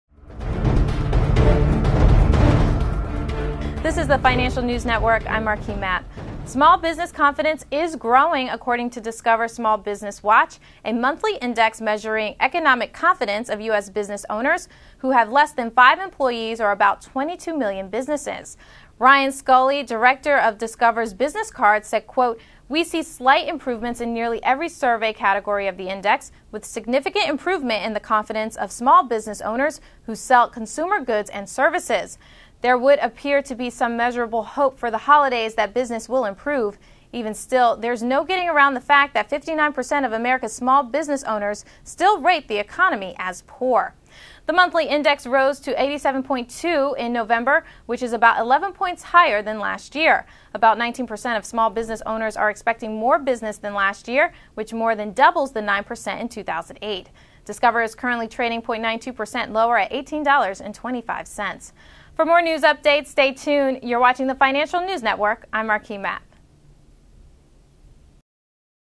Small Business Confidence Growing
"Small business confidence is growing, according to Discover (NYSE:DFS) Small Business Watch, a monthly index measuring economic confidence of U.S. business owners who have less than five employees, or about 22 million businesses.

""We see slight improvements in nearly every survey category of the index, with significant improvement in the confidence of small business owners who sell consumer goods and services,"" said Ryan Scully, director of Discover's business card. ""There would appear to be some measurable hope for the holidays that business will improve. Even still, there's no getting around the fact that 59 percent of America's small business owners still rate the economy as poor.""

The monthly index rose to 87.2 in November, which is about 11 points higher than last year.

About 19 percent of small business owners are expecting more business than last year, which more than doubles the 9 percent in 2008.

Discover is currently trading 0.92 percent lower at $18.25."